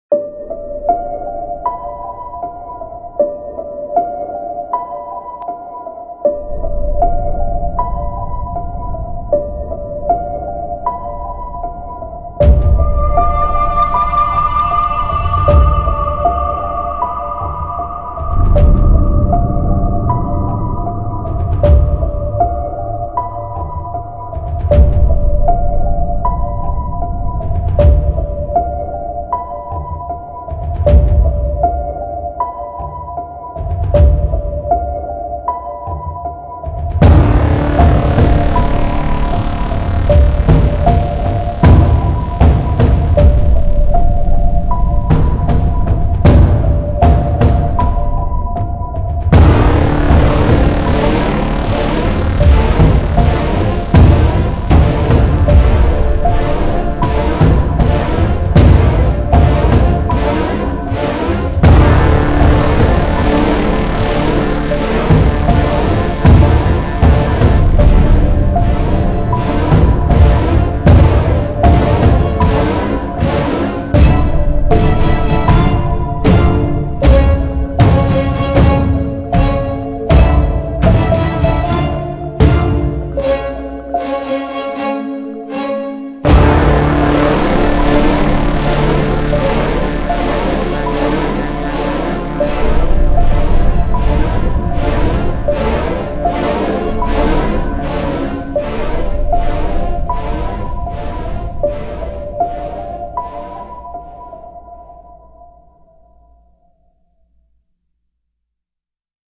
Gromus' Night Terrors 3
A video of my paintings with music featuring my character Gromus. Gromus' Night Terrors 3.